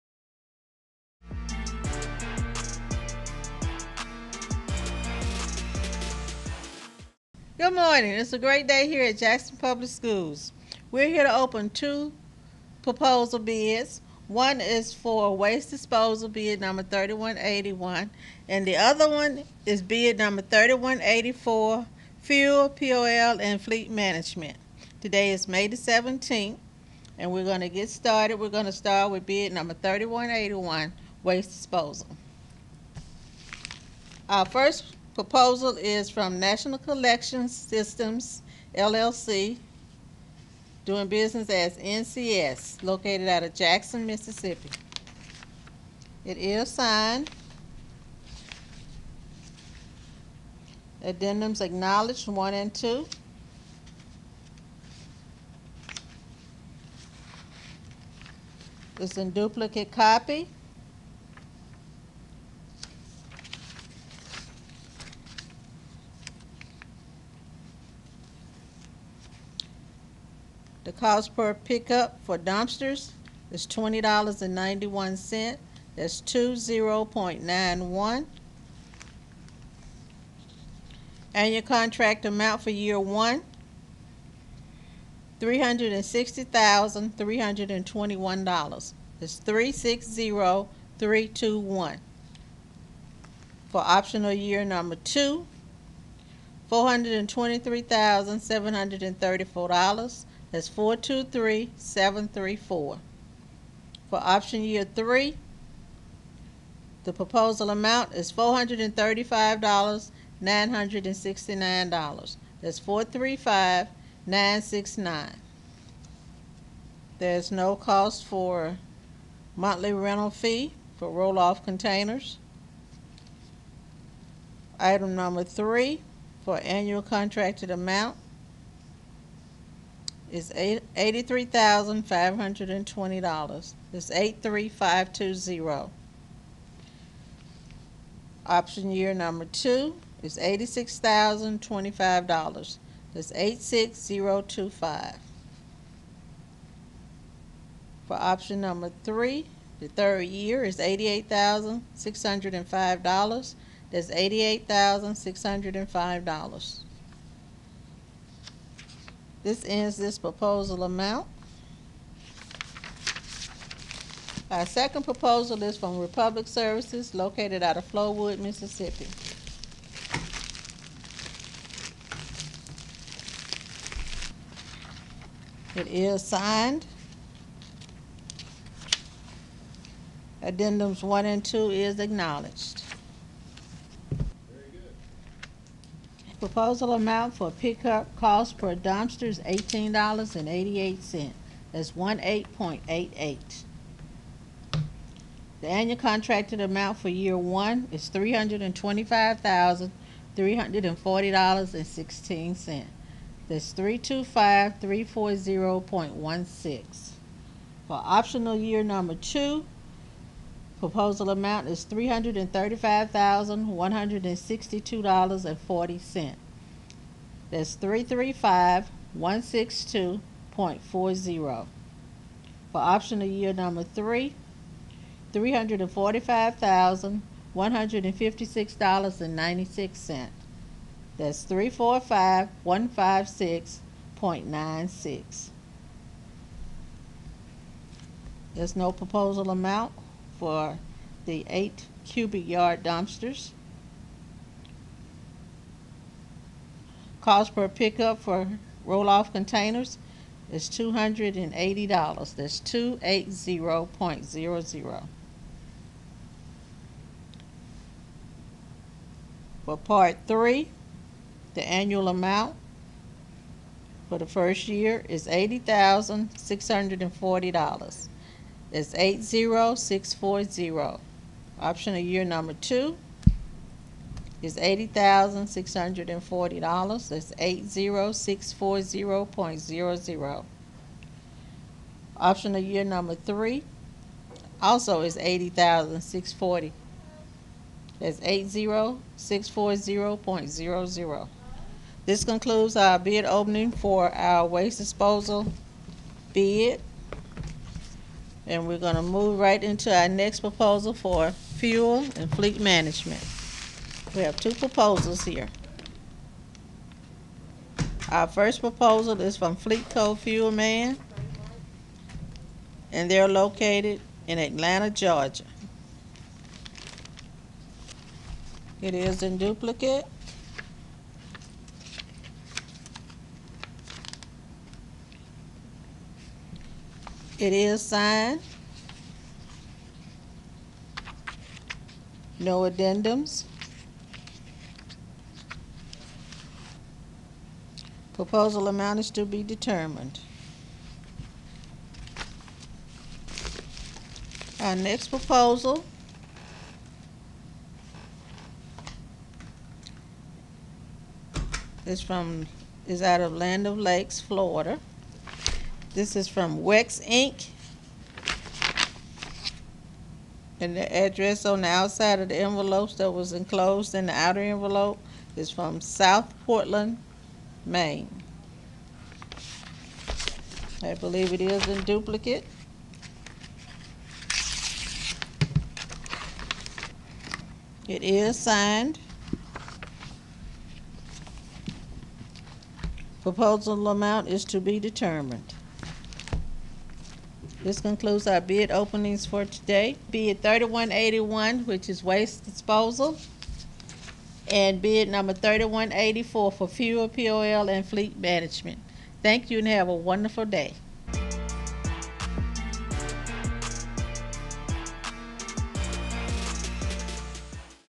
JPS Bid Opening. #3161, #3184. Waste disposal, Fuel, POL & Fleet Mgmt. 5/17/21.
JPS Bid Opening. #3161, #3184. Waste disposal, Fuel, POL & Fleet Mgmt.,  Jackson Public School District, Jackson, MS.  5/17/21.